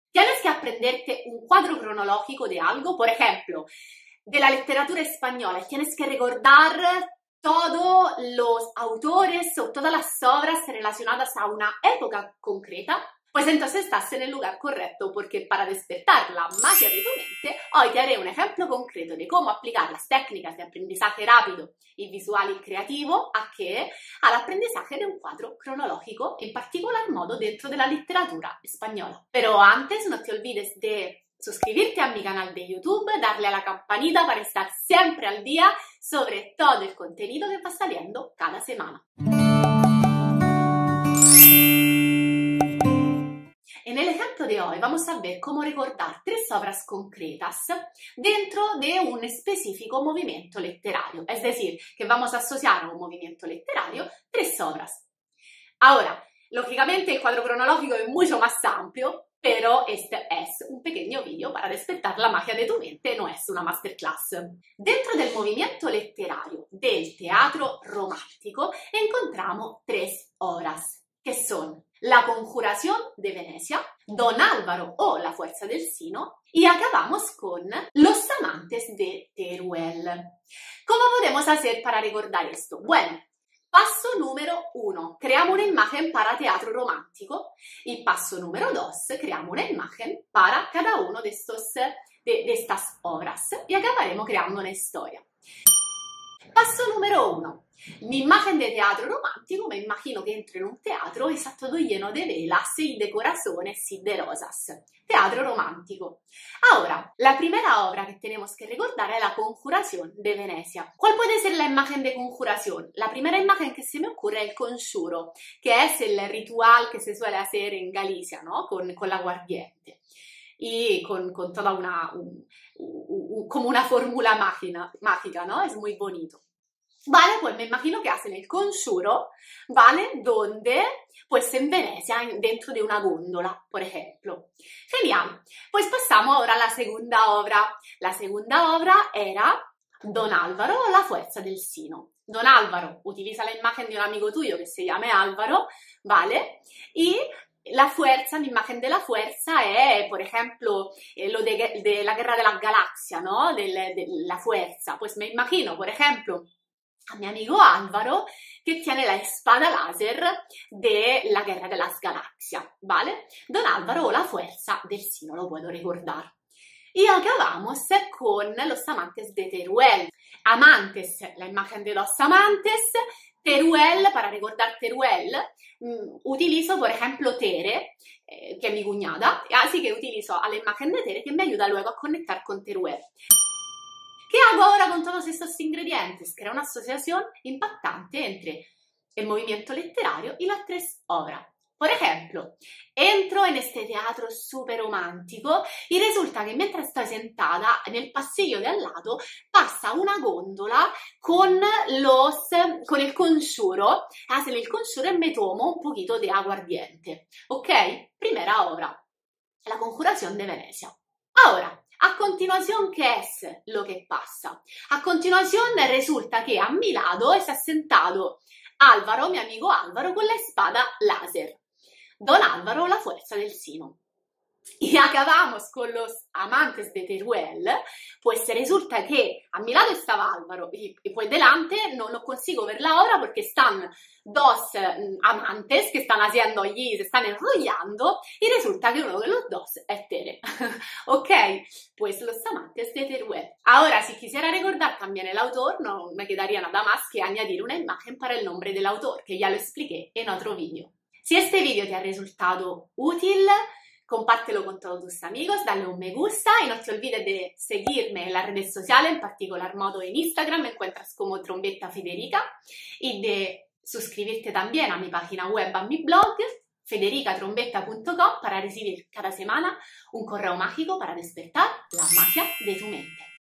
Cómo memorizar un cuadro cronológico
En este video voy a explicarte cómo memorizar obras relacionadas con un específico movimiento literario dentro del cuadro cronológico de la literatura española.